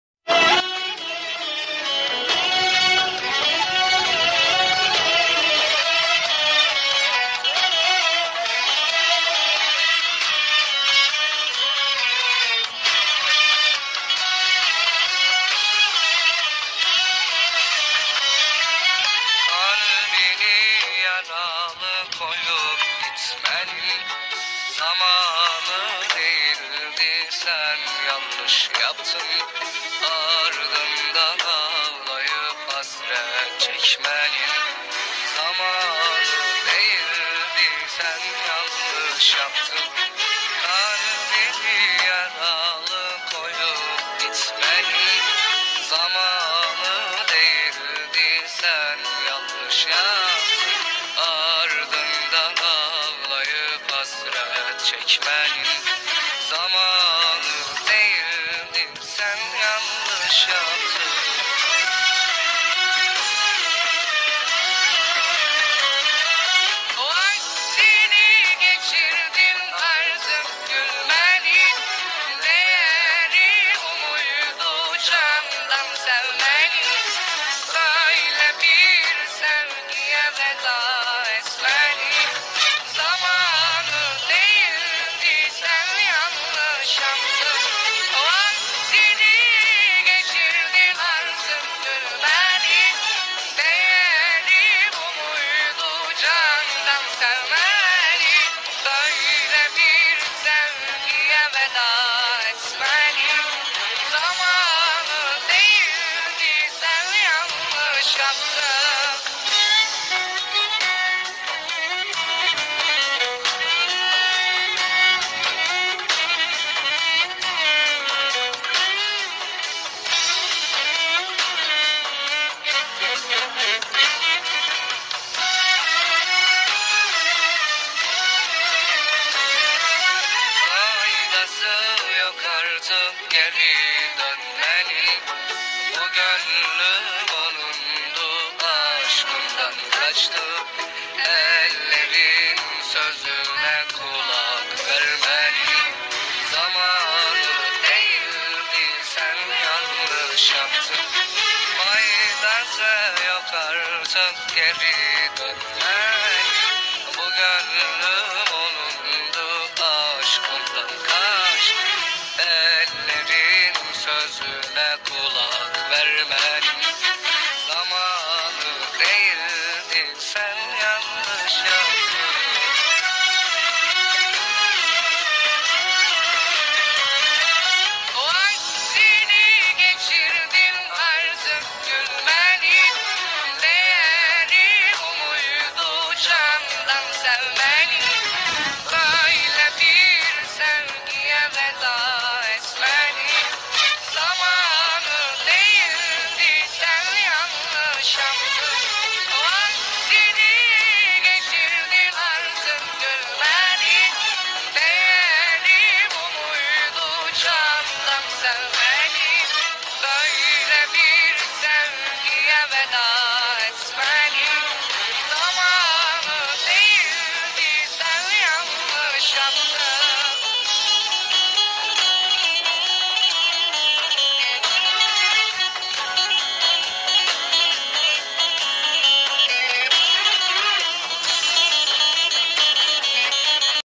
sahin kendirci ile Nargile Keyfin dibine vurmak ! Vlog
Bahcede keyf yapmaktayiz !